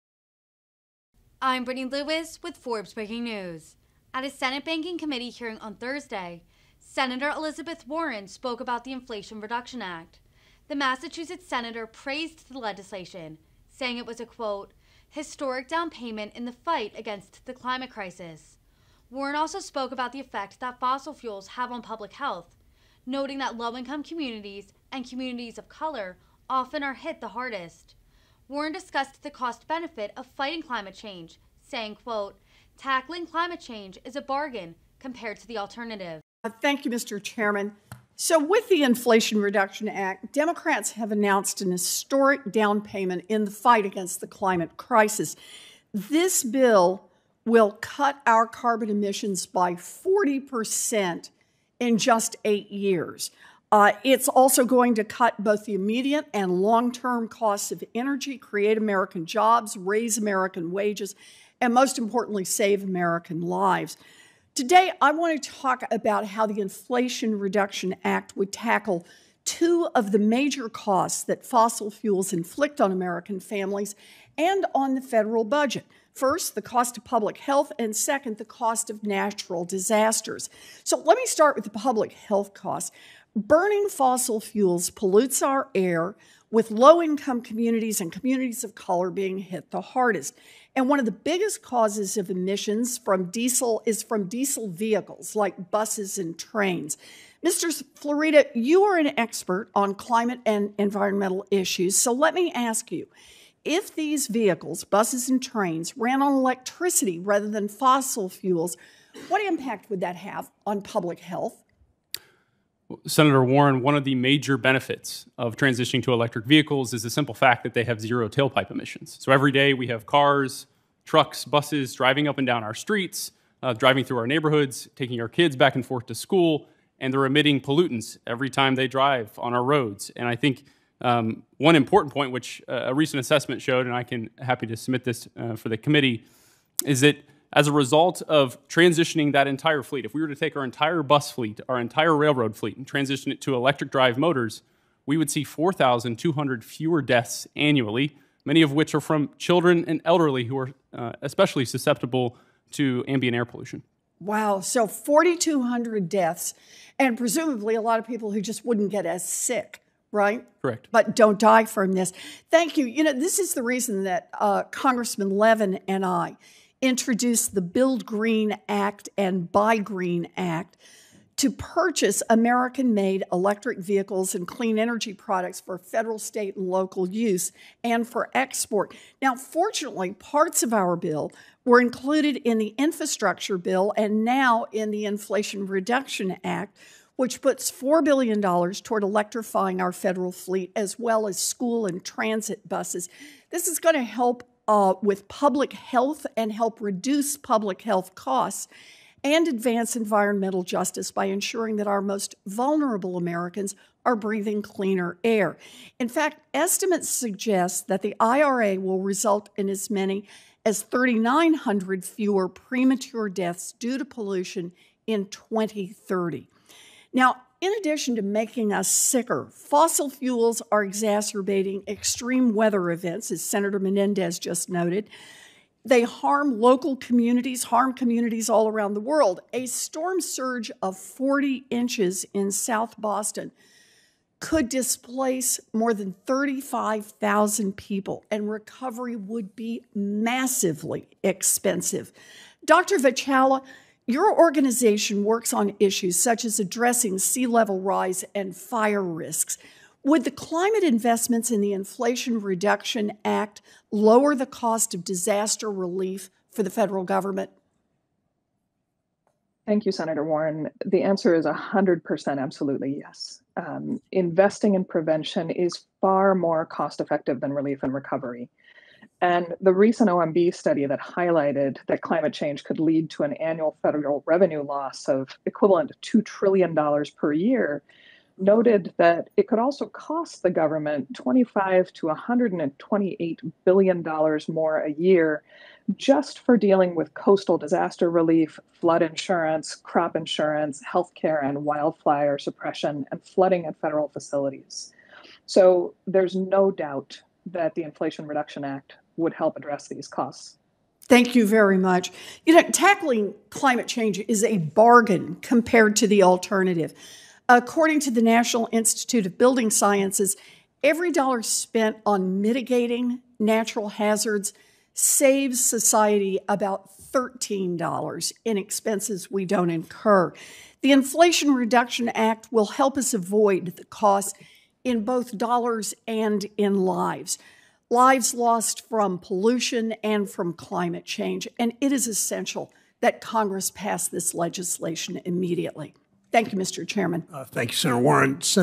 Elizabeth Warren: 'Tackling Climate Change Is A Bargain Compared To The Alternative'
At a Senate Banking Committee hearing on Thursday, Sen. Elizabeth Warren (D-MA) spoke about climate change.